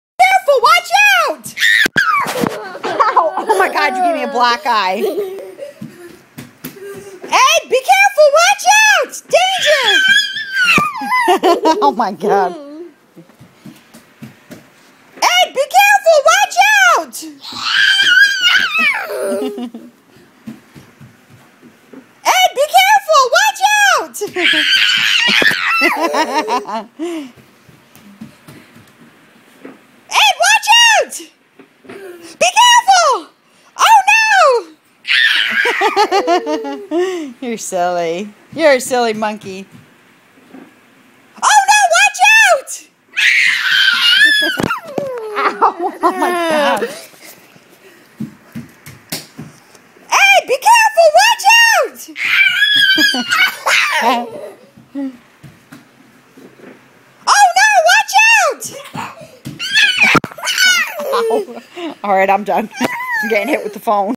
Ed be careful
Eddie having fun with mom telling him to Be Careful!